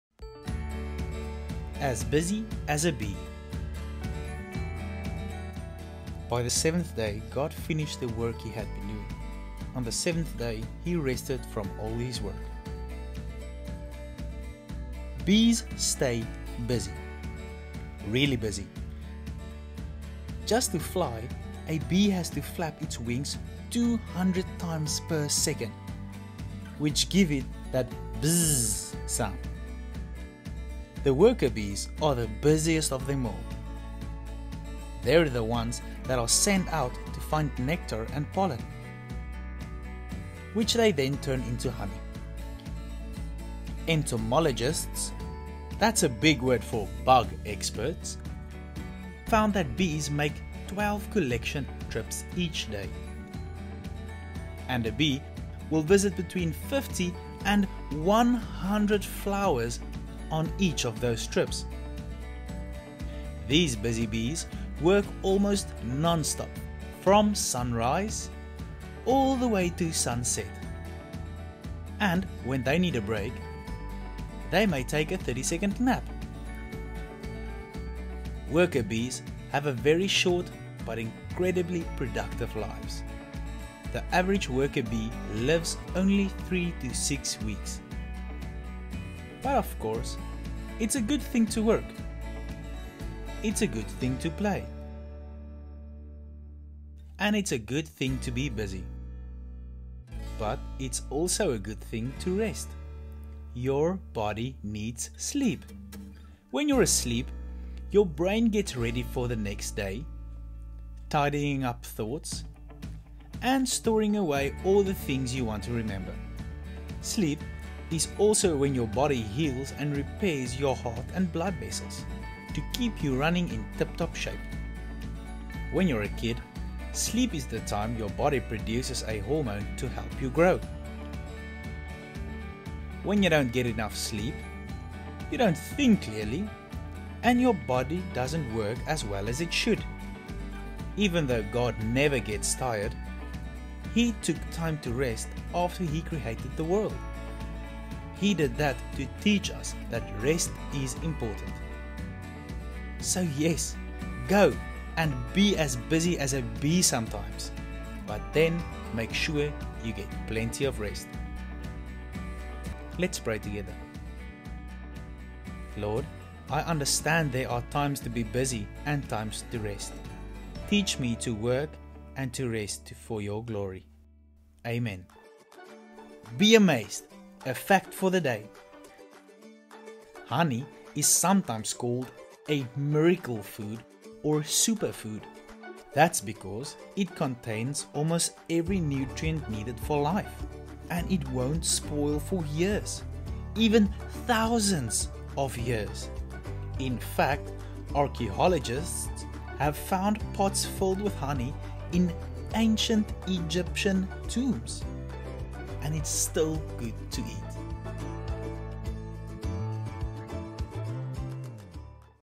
HARTKLOP Wow Series - As Busy As A Bee
As busy as a bee... There are times to be busy and there are times to rest. Our bodies need rest to function properly. Learn to work and rest to God's glory!